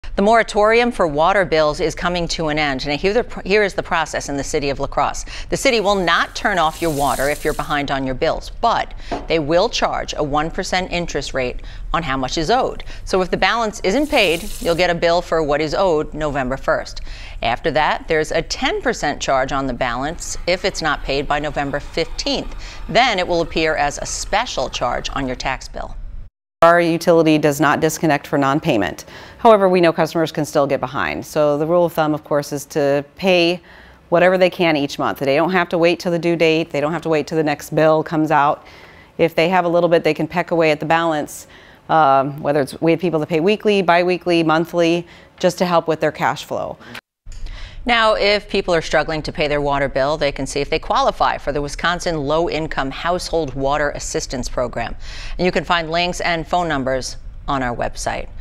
Moratorium on water utilities coming to an end
With the moratorium on water bills coming to an end there are a few things that the City of La Crosse wants you to know about if you're behind on your water bill.